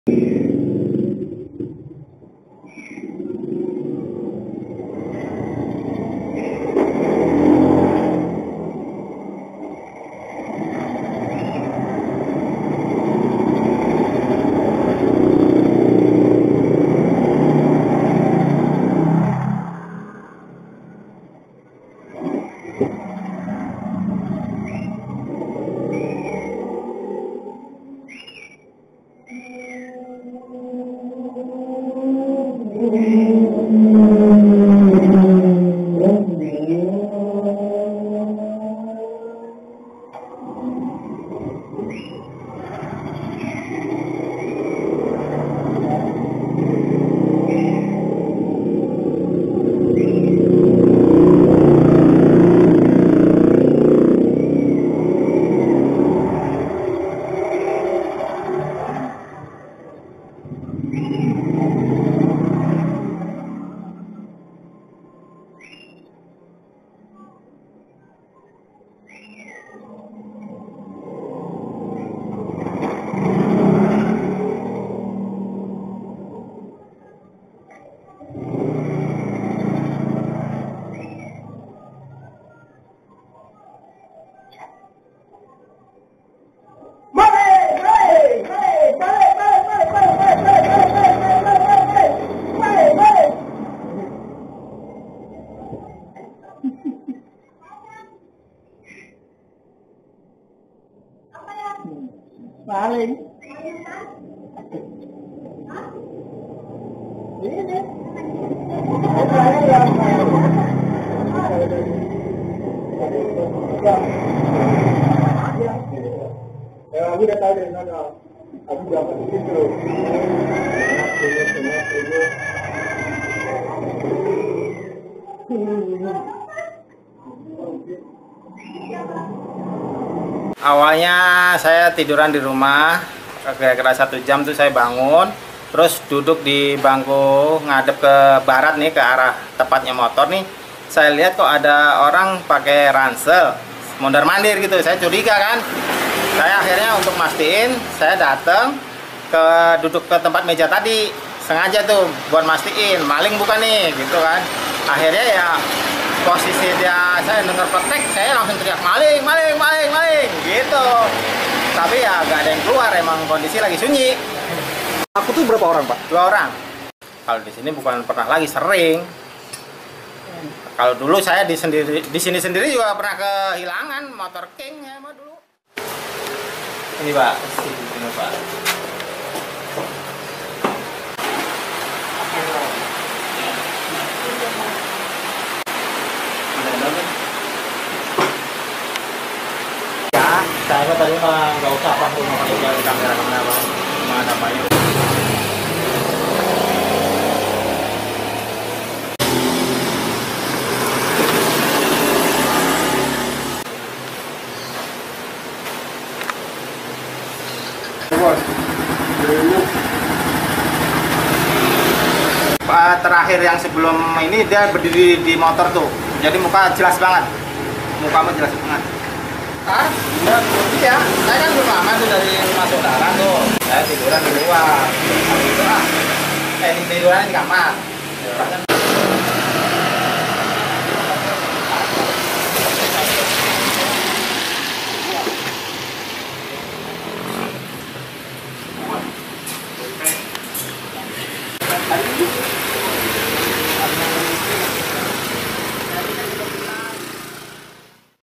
Percobaan Maling Motor Digagalkan Pemilik di Pondok Aren
Aksi percobaan pencurian motor yang terjadi di kawasan Jalan Palem Puri, Pondok Aren, Kota Tangerang Selatan (Tangsel) berhasil digagalkan sang pemilik.

Sang pemilik yang bernama Juwanto (43) mengatakan aksi percobaan pencurian itu terjadi pada Senin (11/4/2022).

Menurutnya saat itu motor miliknya sedang terparkir di teras rumahnya.

Kemudian datang dua orang yang tak dikenal menghampiri motornya tersebut yang sedang terparkir tepat di teras rumahnya.

"Motor lagi di teras saya lagi duduk di depan rumah sambil melihat motor. Terus ada dua orang bawa ransel menghampiri motor saya," kata Juwanto